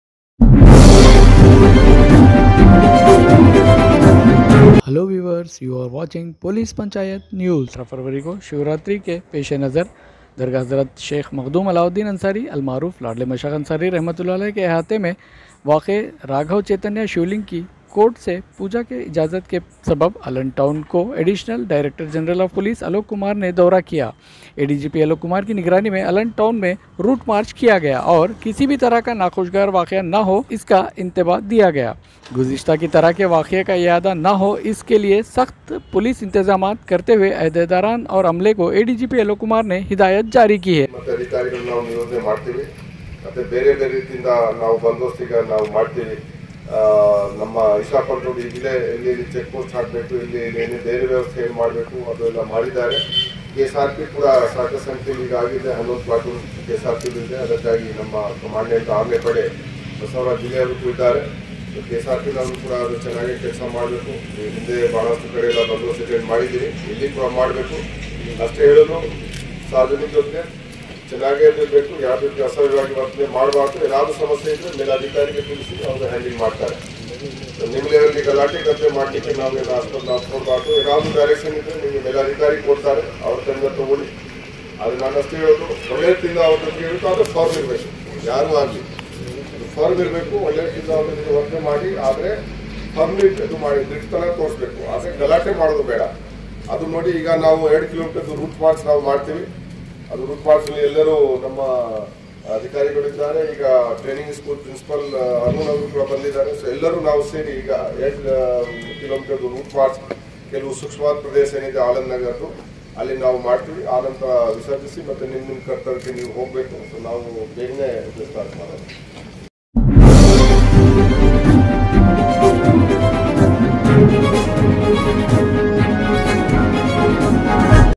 ADGP Alok Kumar IPS visited Aland Town ahead of Maha Shivratri
Police panchayat PPN news network,
Watch the latest News updates